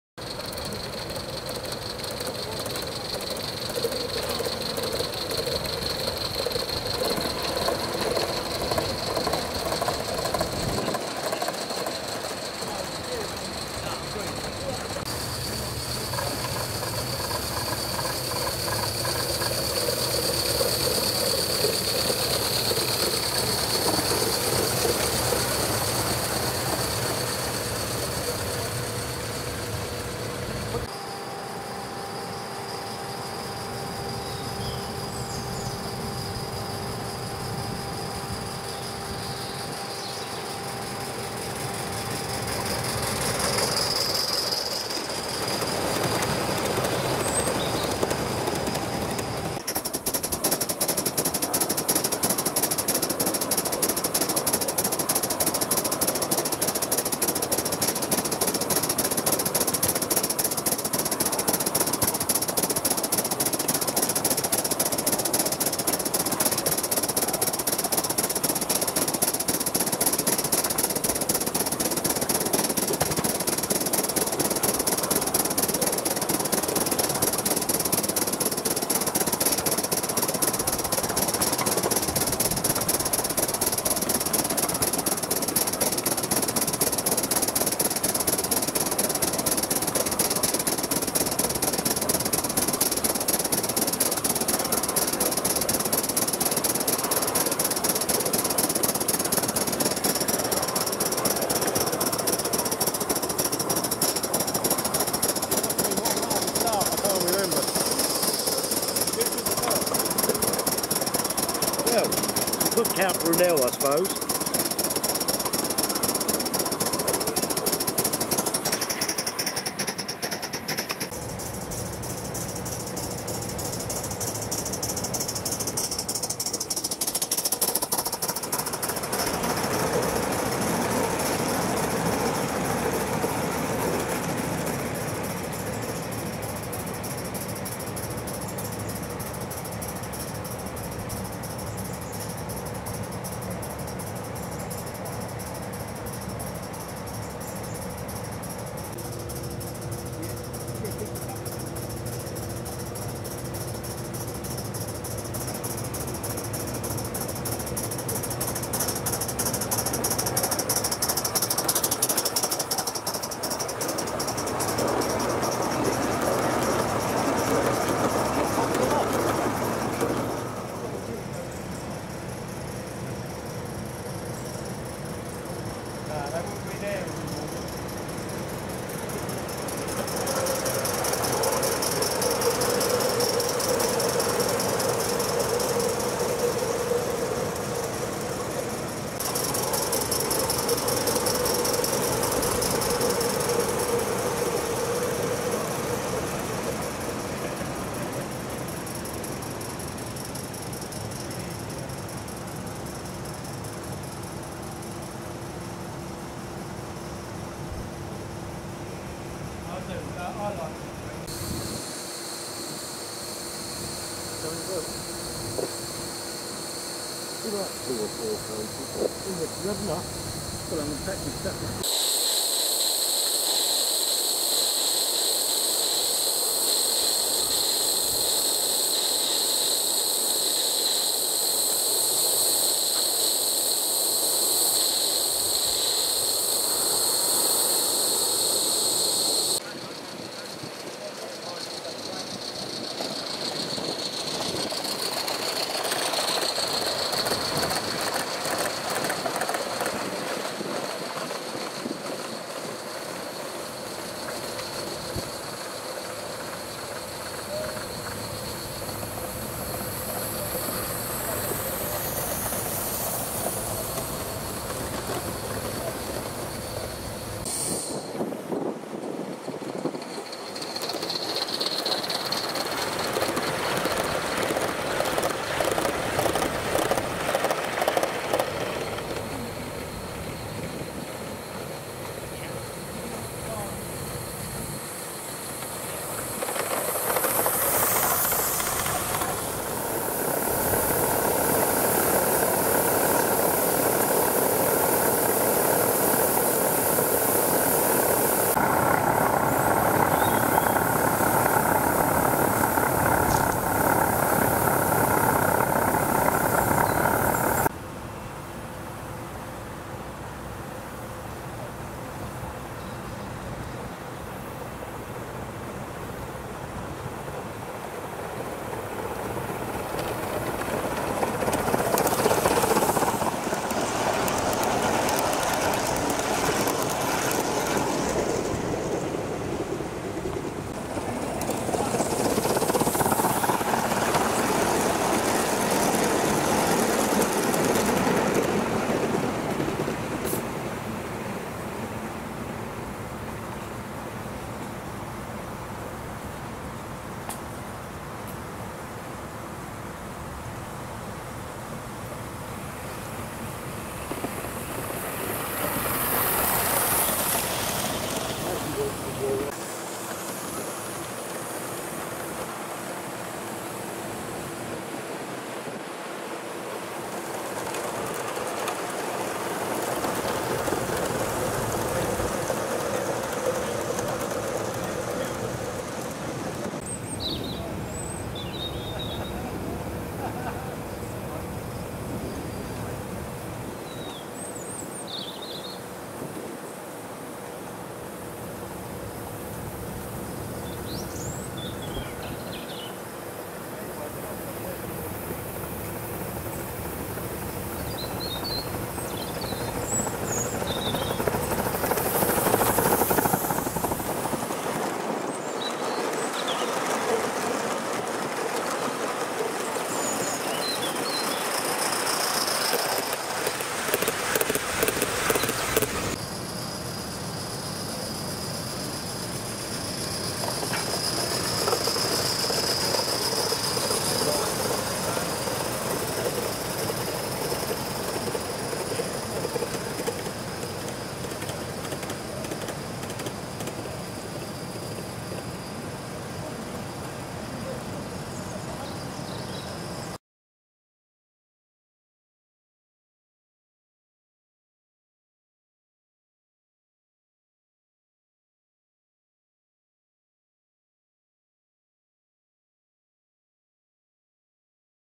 Steam Power at our Club
A coal fired King ,Britannia and more on my short video at our local club